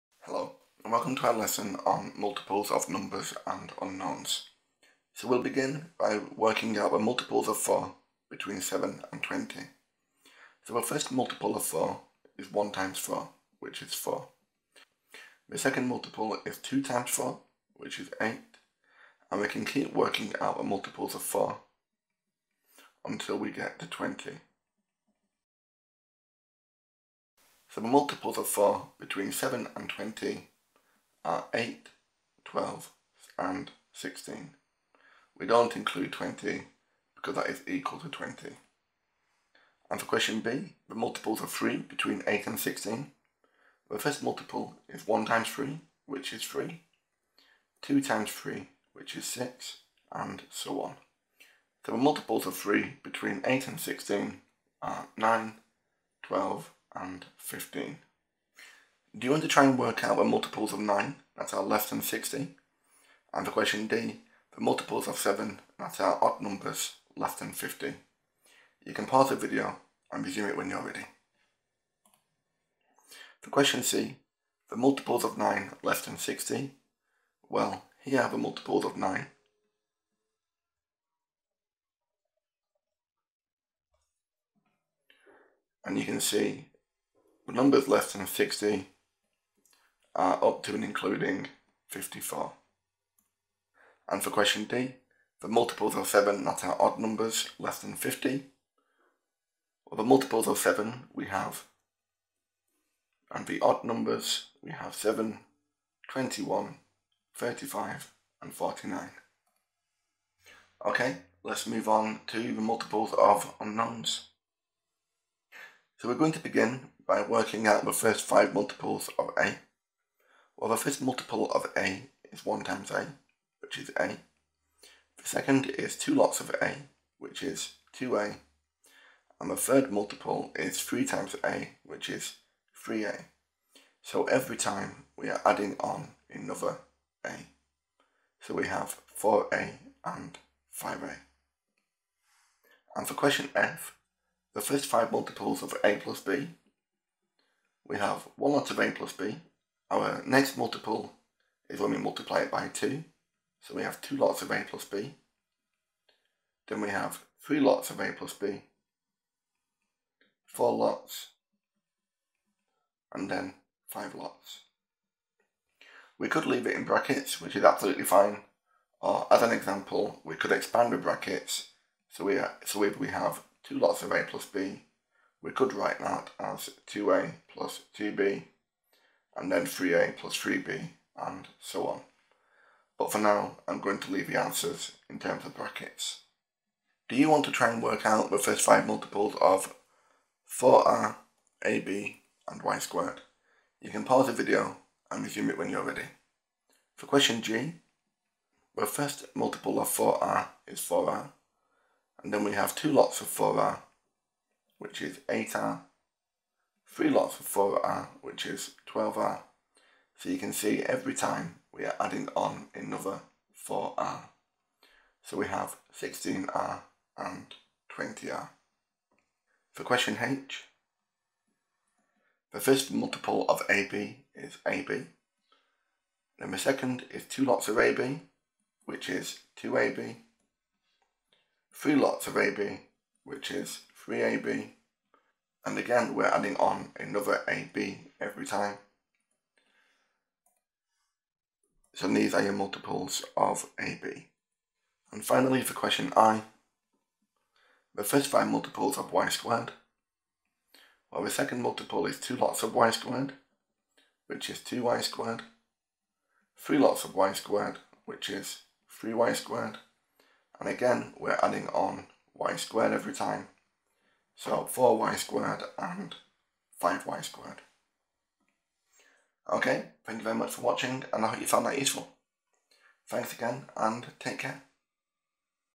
Multiples of Numbers and Terms
How to calculate the multiples of a number and algebraic expression

The full lesson includes a starter, 2 mains, plenary and a worksheet.  Members of Mr Mathematics can download this